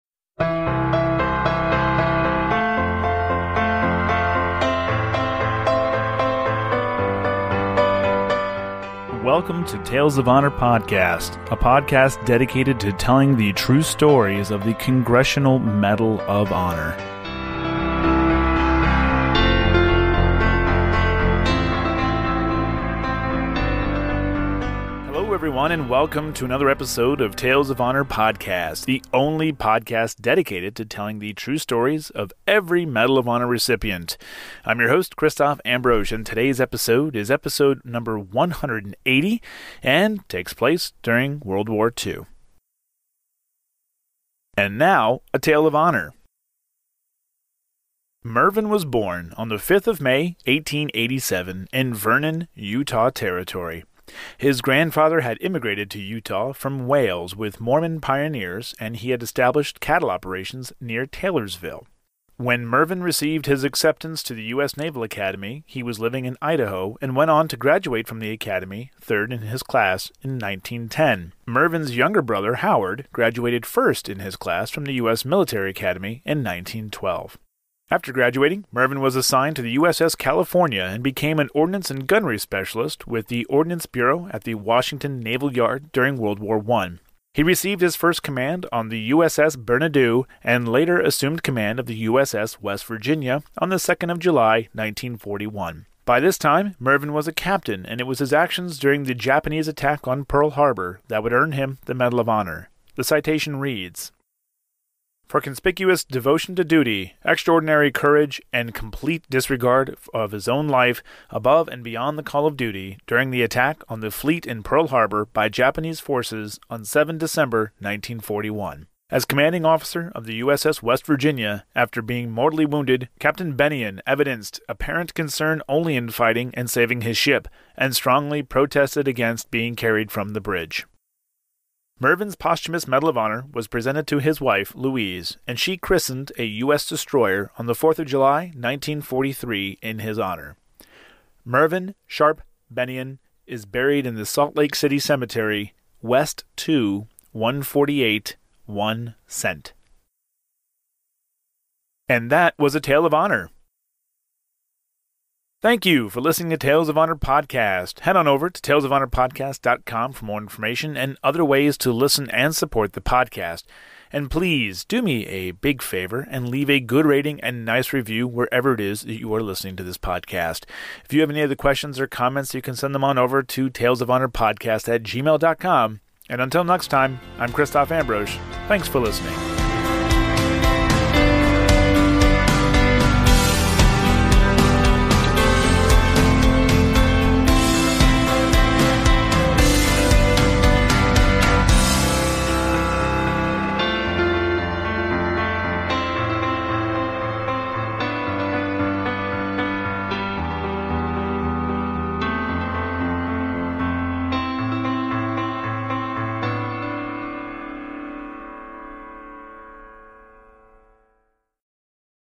Mervyn S Bennion - Medal of Honor Recipient
On episode one hundred and eighty, the story of Mervyn S Bennion is told.

 All stories in February and March will be from World War 2.